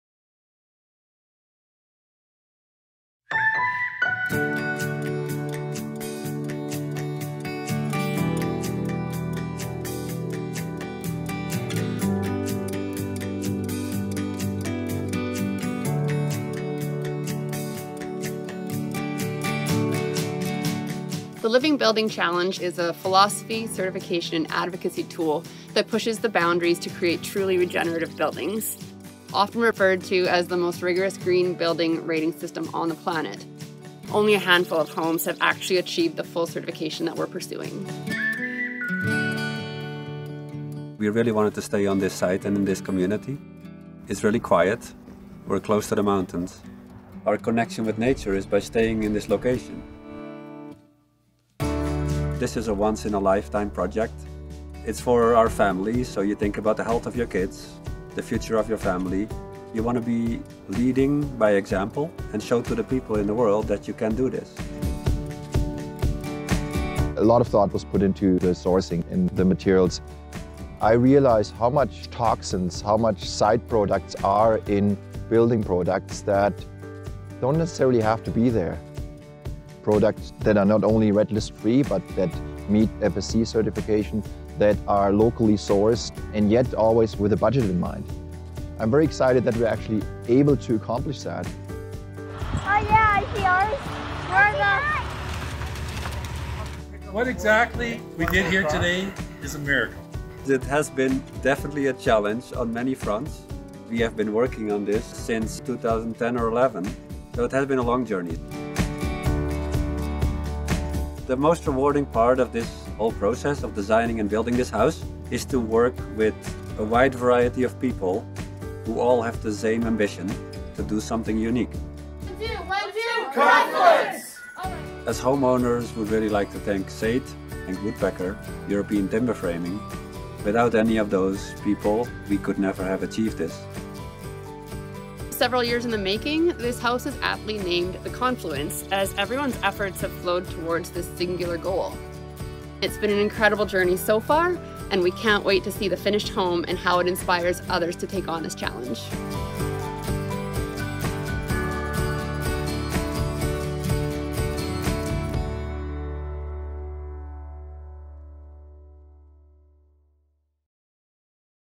Building one of the greenest homes on earth
A house like no other is taking shape in the foothills of southern Alberta. One that produces more energy than it uses, captures water on-site and is anticipated to achieve full certification through the Living Building Challenge — the world’s most rigorous green building rating system and sustainable design framework.

Interested in SAIT's School of Construction?